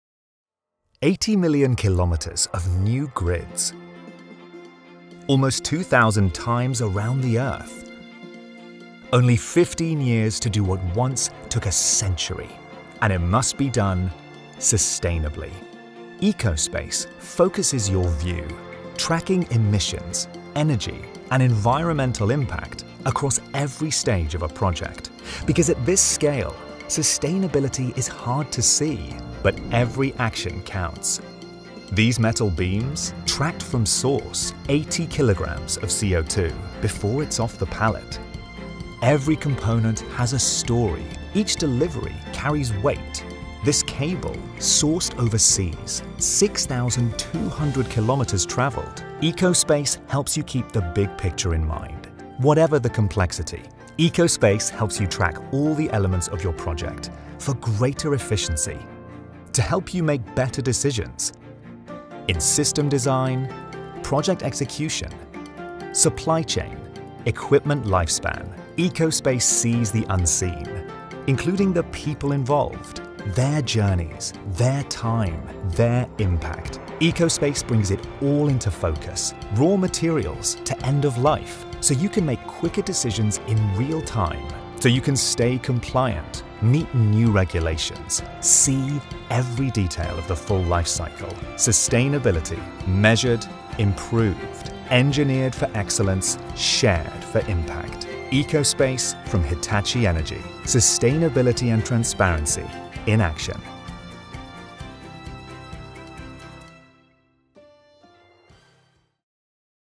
EcoSpace™
EcoSpace™ is Hitachi Energy’s digital platform that delivers fast, certified insights on the environmental impact of large scale grid infrastructure projects. Covering every project element and reporting across the full energy value chain, it enables customers to make carbon-conscious decisions based on trusted data – supporting compliance, financing, and sustainability goals.